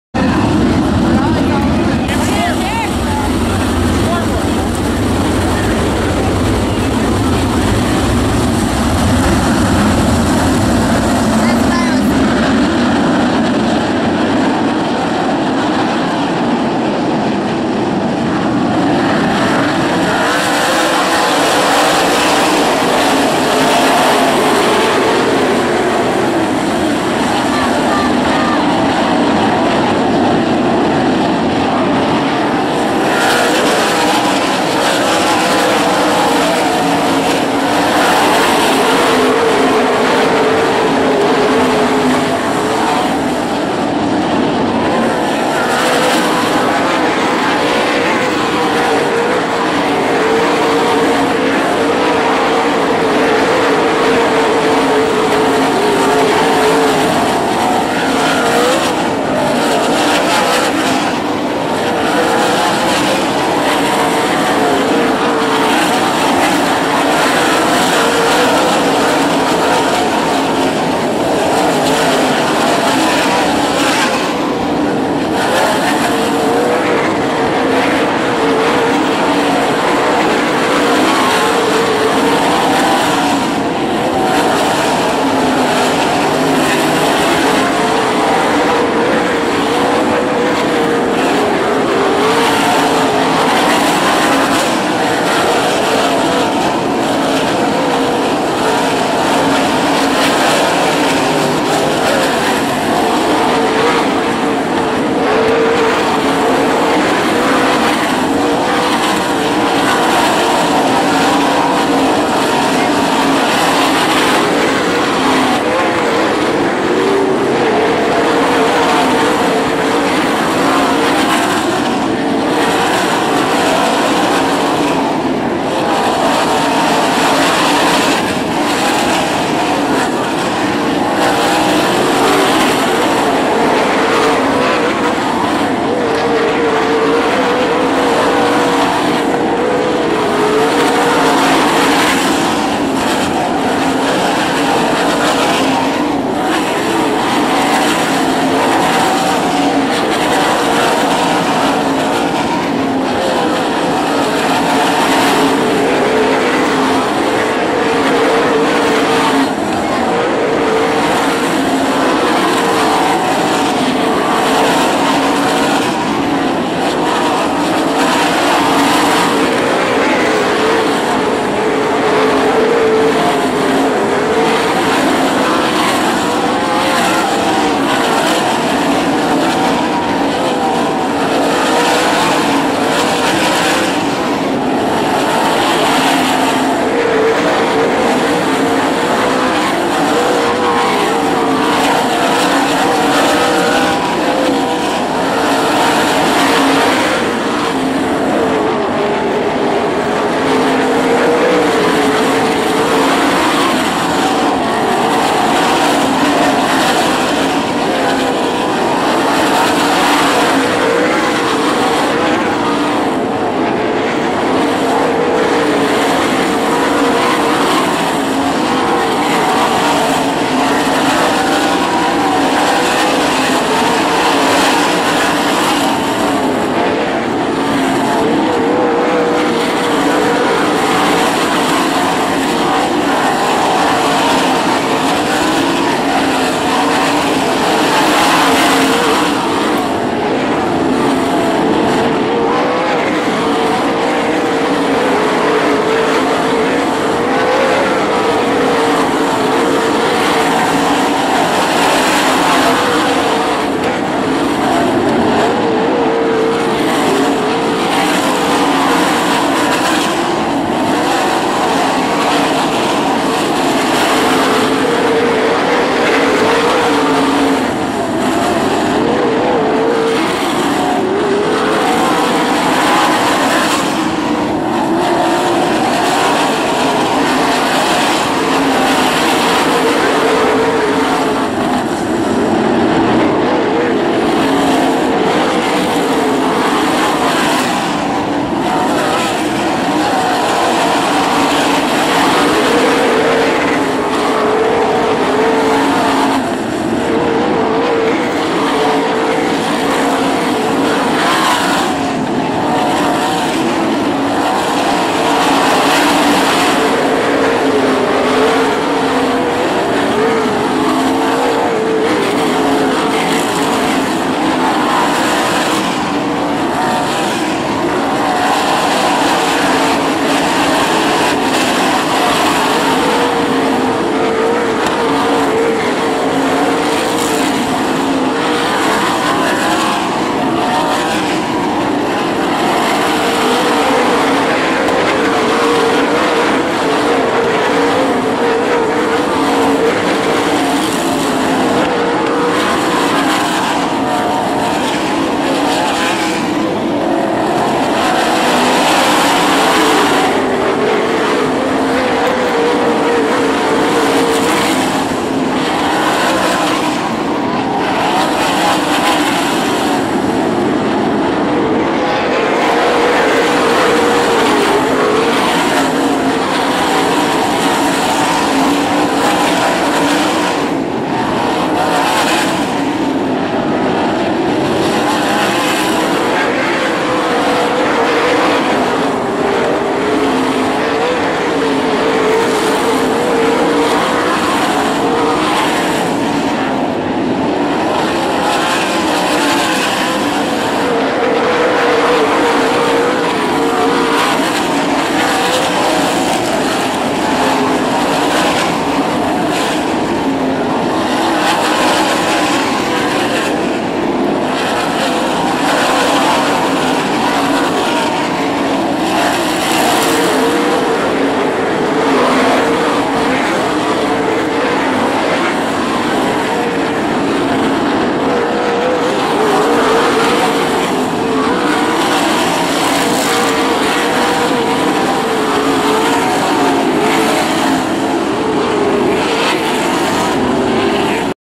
IRA Sprintcar Feature Beaver Dam Raceway 9/21/13
17 Balog 16 Whitney 5 Schultz 18 Madsen 1 Mock